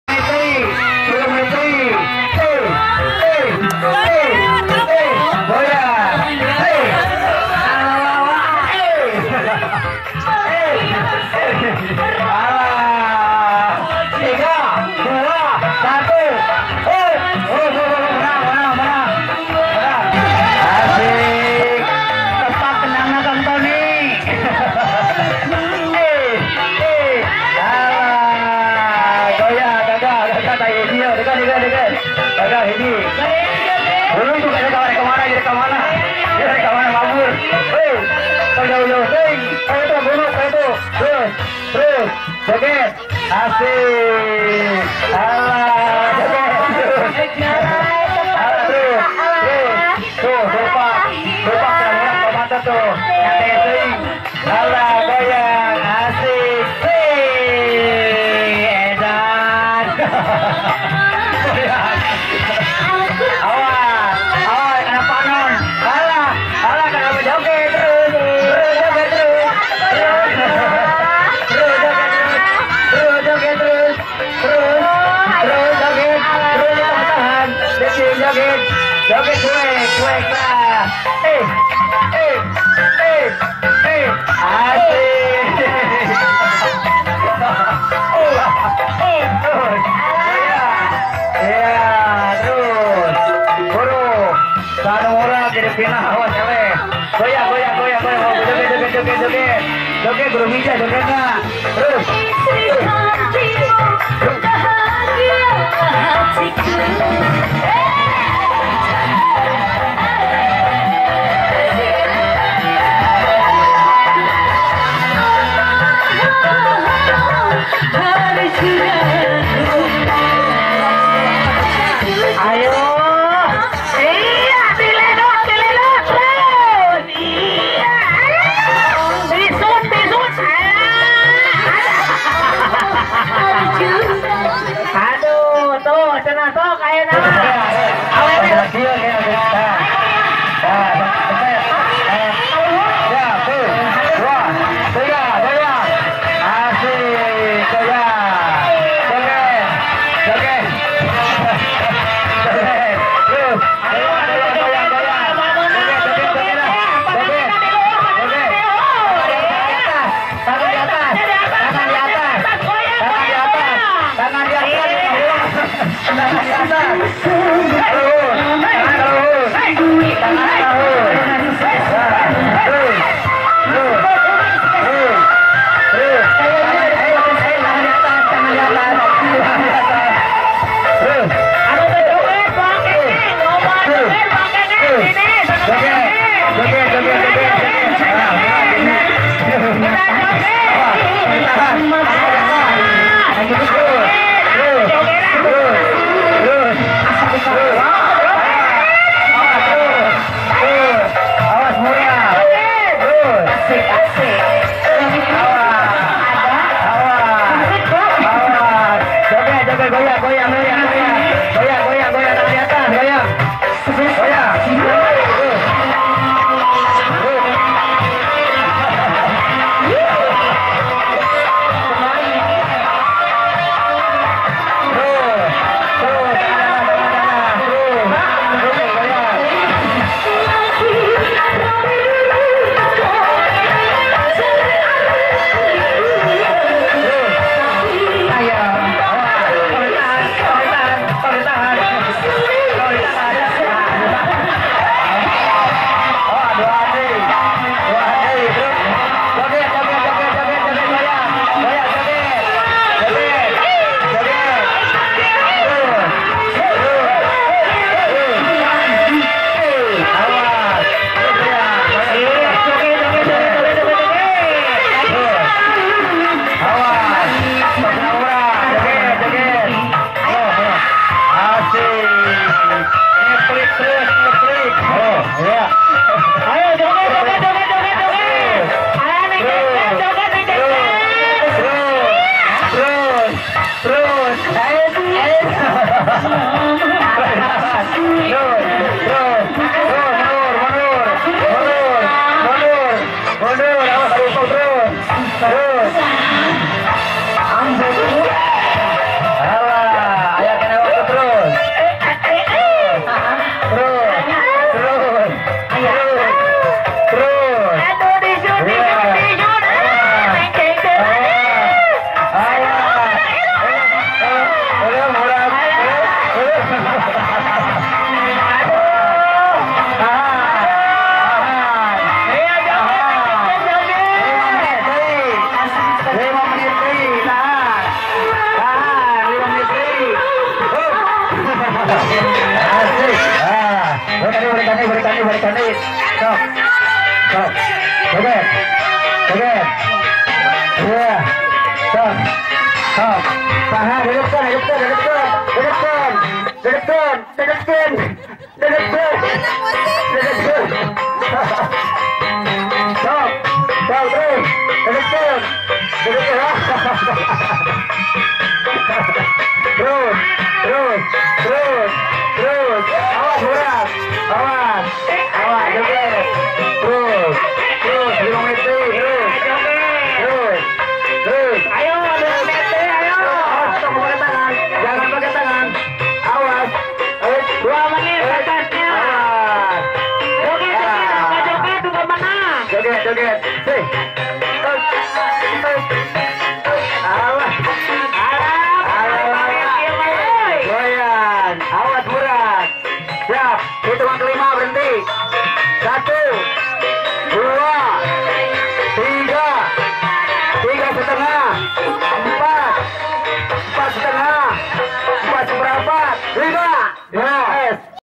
Super lucu; lomba joget anak-anak kampung Cipeuyeum
Dalam memeriahkan HUT RI ke 71, RW 07 Desa Cipeuyeum mengadakan lomba permainan anak-anak, ibu-ibu, dan pemuda